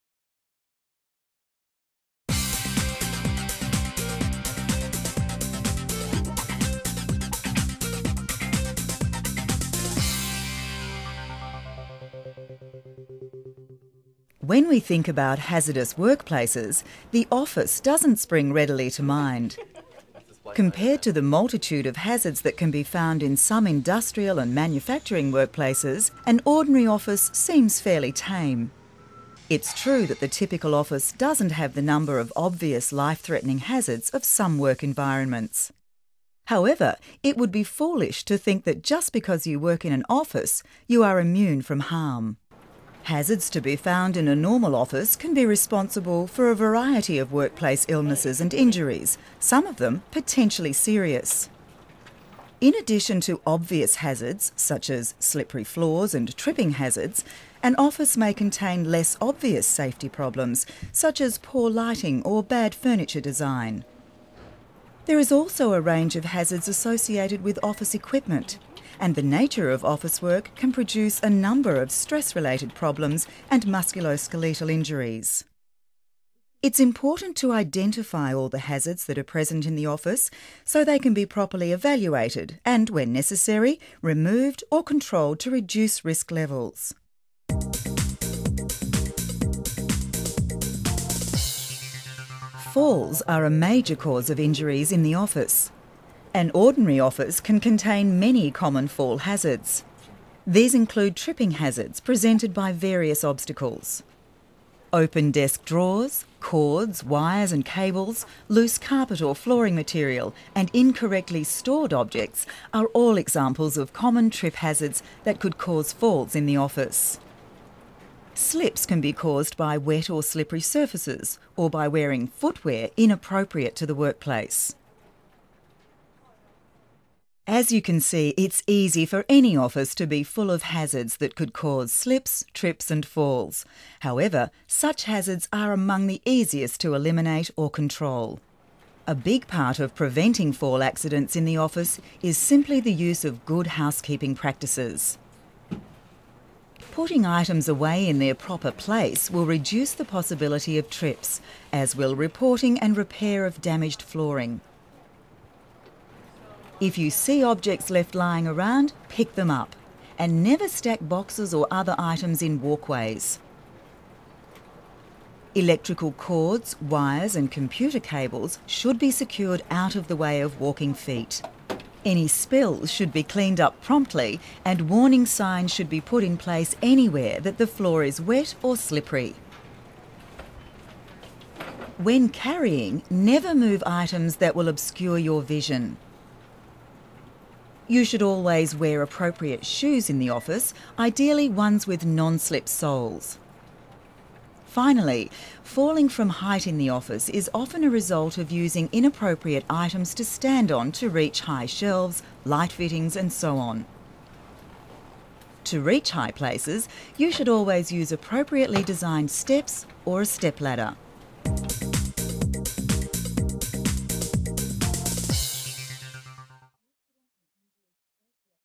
Office Safety DVD - Safety training video for the office - Safetycare free preview
This program has been produced to inform and educate on the safety issues that exist in the office environment. It looks at the common and often overlooked hazards that could exist in an average office, and how these hazards can be recognised and controlled to reduce the risks to individuals working in offices.

The program SAFETY IN THE OFFICE provides:

- An understanding that although the typical office doesnt contain the obvious life-threatening hazards of some work environments, the hazards that do exist in the office can nonetheless cause real damage

- An awareness of the different kinds of hazard that exist in the office, such as slip, trip and fall hazards, manual handling hazards, fire hazards and electrical hazards

- An understanding of the principles behind workstation ergonomics, and how these principles can be used to avoid injuries and protect health

- Knowledge of the correct steps to be taken to avoid accidents and injuries in the office environment, such as good housekeeping, correct lifting and carrying, safe use of electricity, and fire prevention  AND fire fighting methods

- A heightened awareness of the issue of workplace safety when applied specifically to the office situation.

Its easy to consider the office a perfectly safe work environment, and forget about the hazards that can exist. Just because you work in an office, it doesnt mean you shouldnt be concerned for your own health and safety, and that of your colleagues.

For the best and most current Safety Video Content - Go